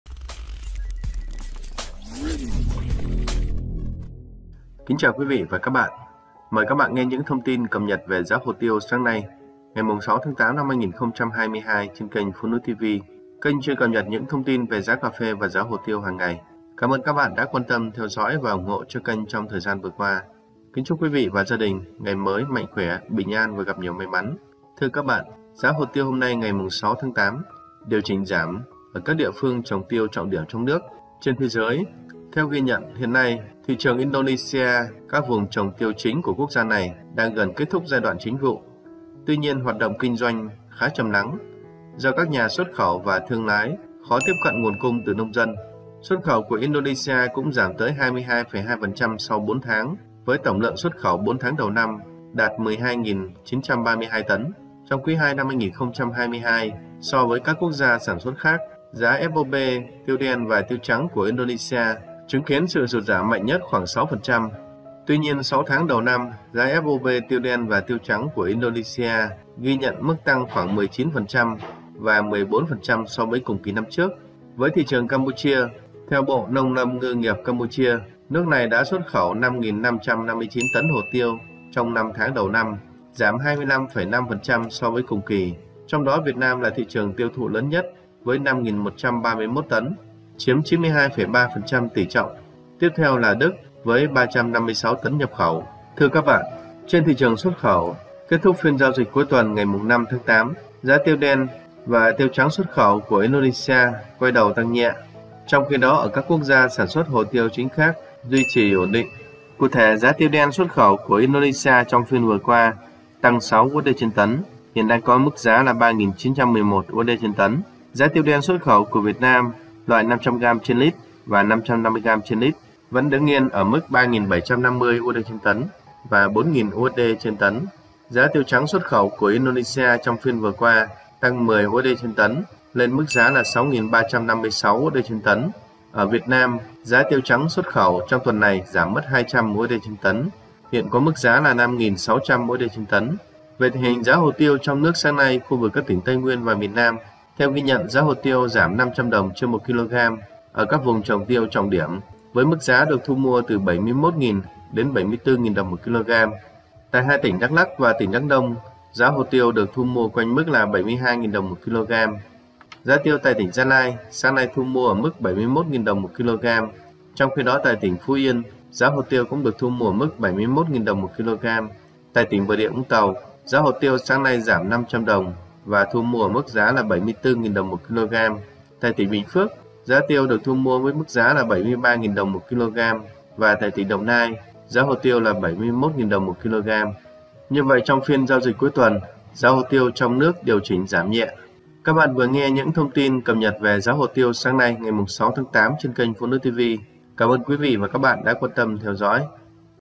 Giá hồ tiêu hôm nay ngày 6/8/2022|Giá tiêu trong nước điều chỉnh ở các địa phương
👉 Bản tin cập nhật giá hồ tiêu hôm nay ngày 6/8/2022 được chúng tôi cập nhật chi tiết, các thông tin trên thế giới và trong nước tại các vùng trồng trọng điểm khu vực Tây Nguyên và miền Nam. Giá chi tiết tại các địa phương như Đắk Lắk, Gia Lai, Kon Tum, Đắk Nông, Lâm Đồng, Đồng Nai, Bình Dương...
© Copyright by PhoNui TV ☞ Do not Reup